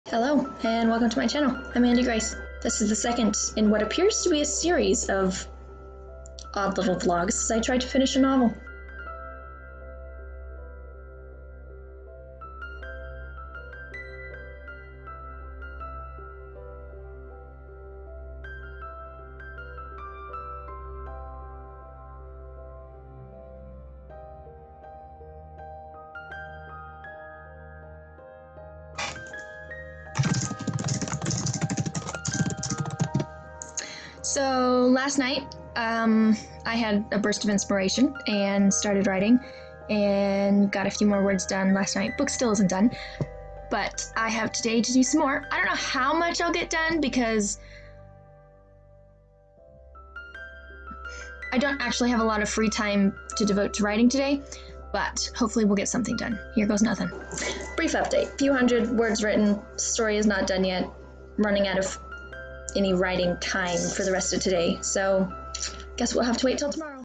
Trying to Finish my WIP Day 2
Obviously I haven't bought a camera between yesterday and today, so still using the sorry little webcam. Have patience with me and my computer.

Also, have written more words...but not enough to finish the novel.